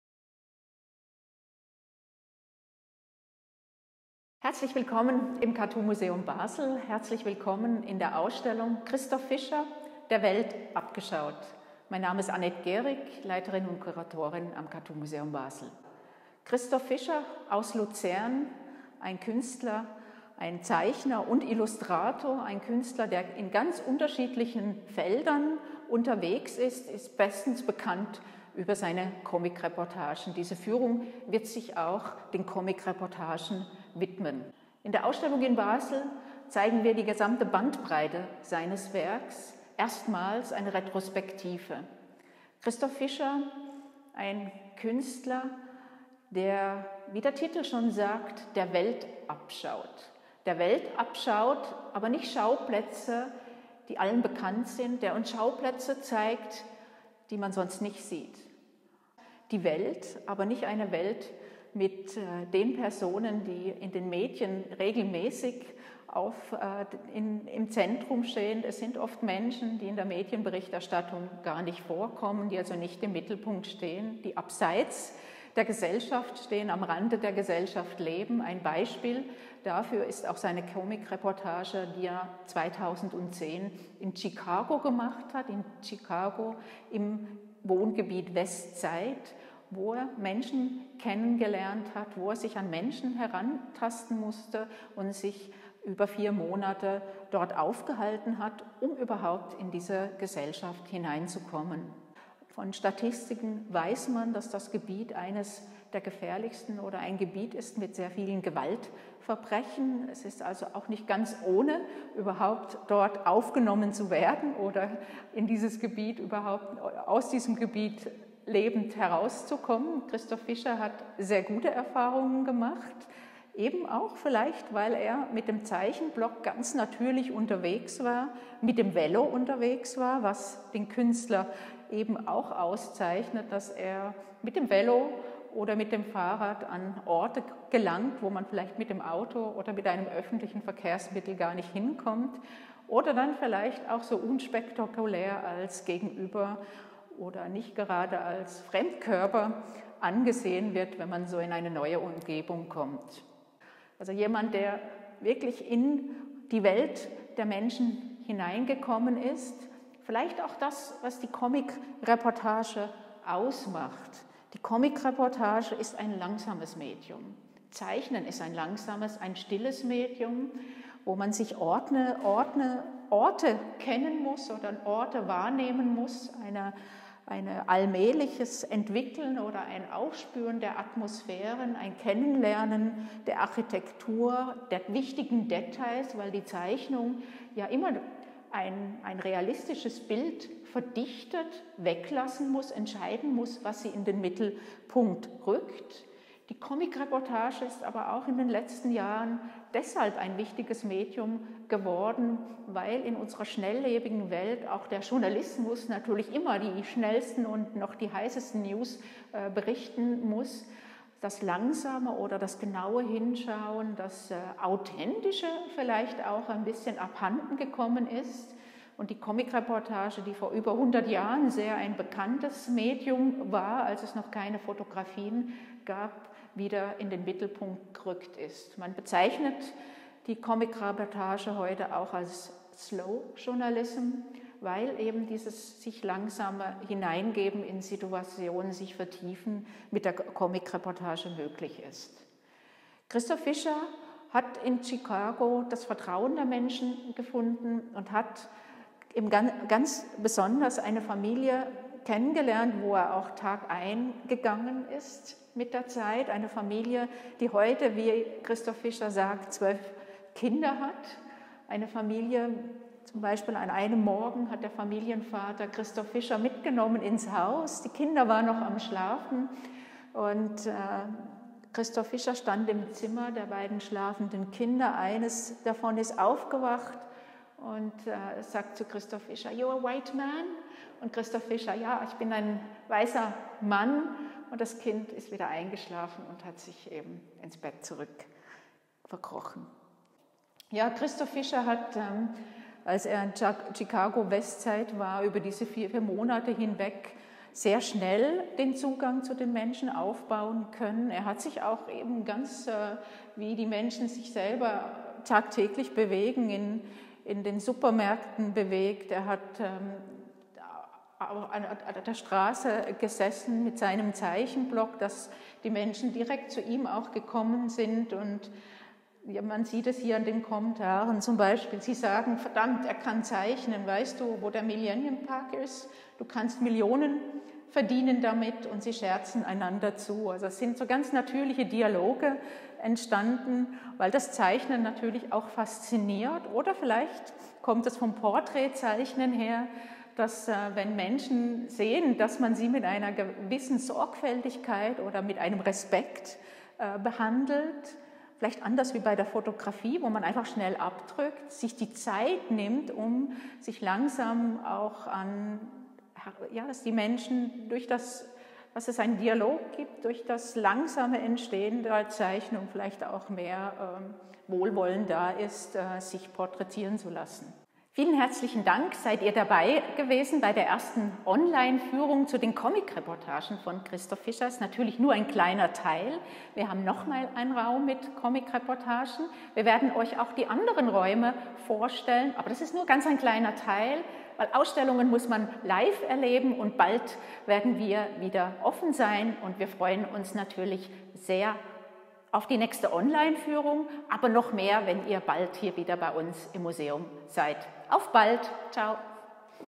Online Kuratorenführung Nr. 1 durch die Ausstellung «Christoph Fischer. Der Welt abgeschaut»
Kuratorin und Direktorin Anette Gehrig spricht über die Comic-Reportage und stellt die Werkserie «Chicago Westside» von Christoph Fischer vor.